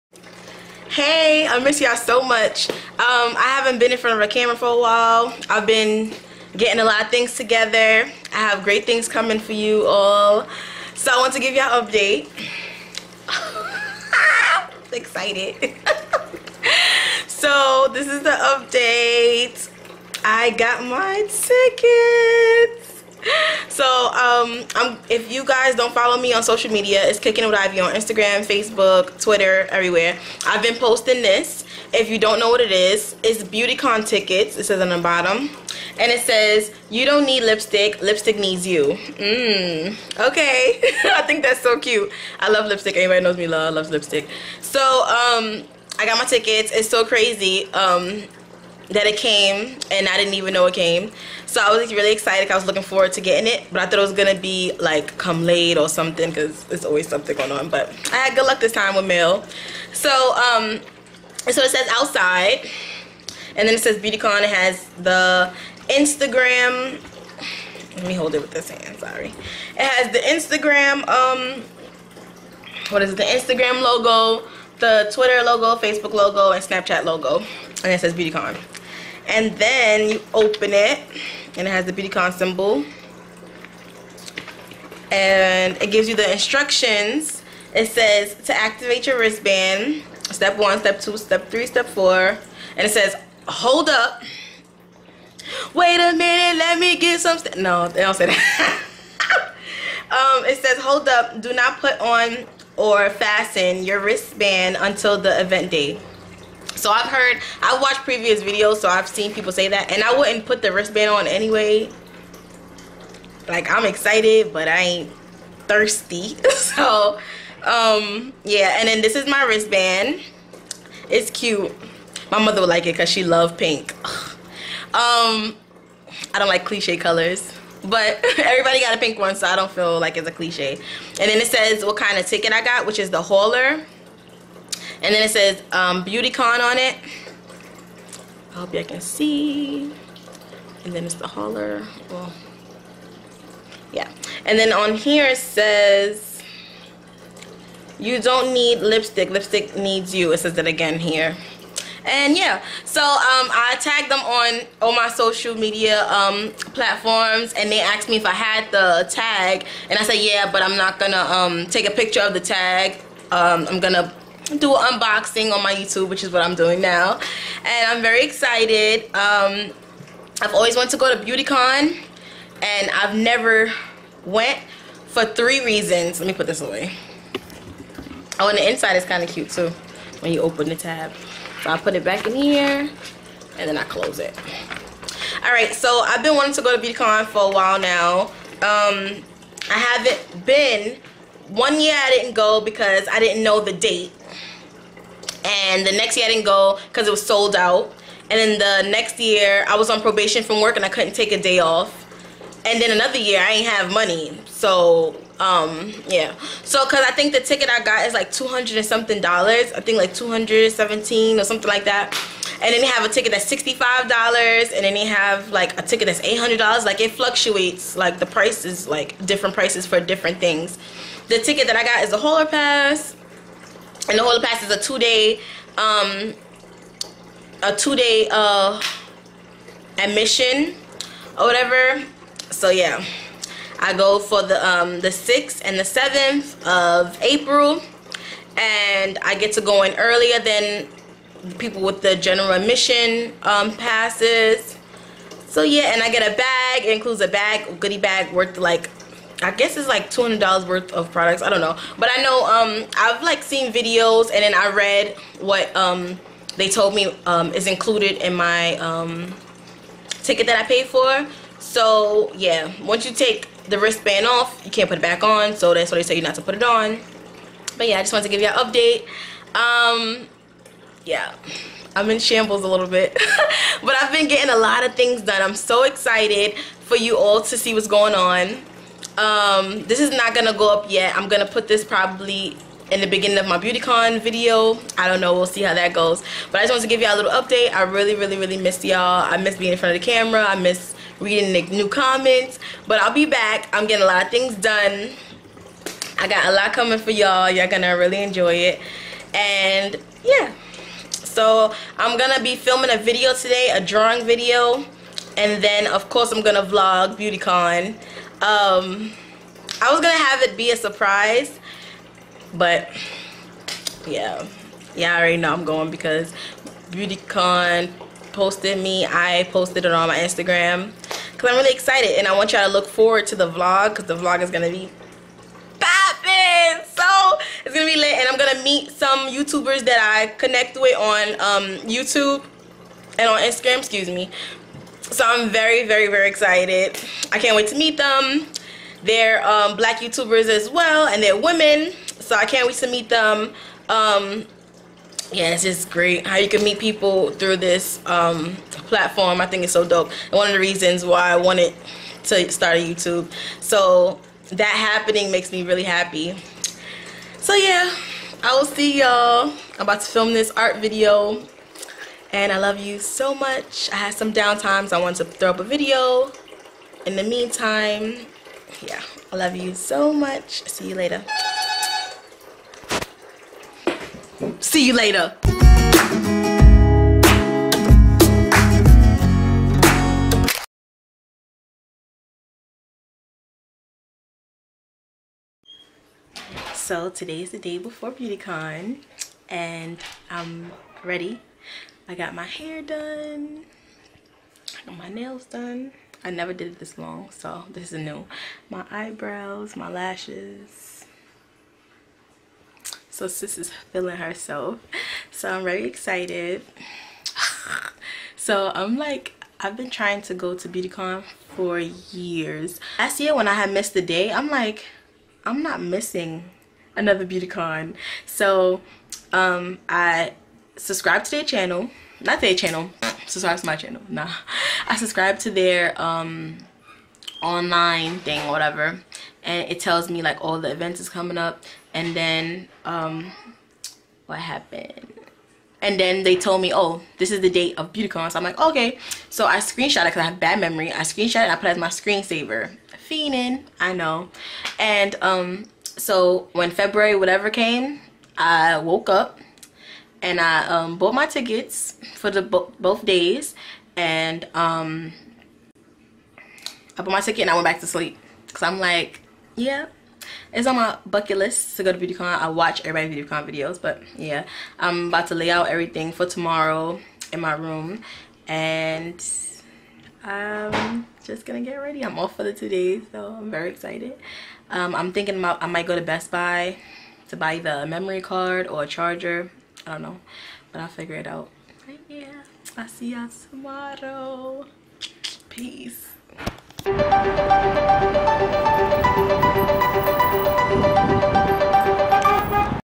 BEAUTYCON TICKET !!
HEY LOVES I MISS YOU ALL SO MUCH. TODAY I AM COMING TO YOU WITH A VIDEO OF WHAT IVE BEEN WAITING FOR IN THE MAIL. IVE BEEN WANTING TO GO TO BEAUTYCON FOR YEARS! IF YOU DONT KNOW WHAT BEAUTYCON IS I WILL BE EXPLAINING IT AND YOU WILL SEE MY REACTION IN THIS VIDEO AS WELL LOVE YOU SEE YOU IN THE COMMENTS

INSTAGRAM KICKIN IT WITH IV
FACE BOOK KICKIN IT WITH IV
TWITTER KICKIN IT WITH IV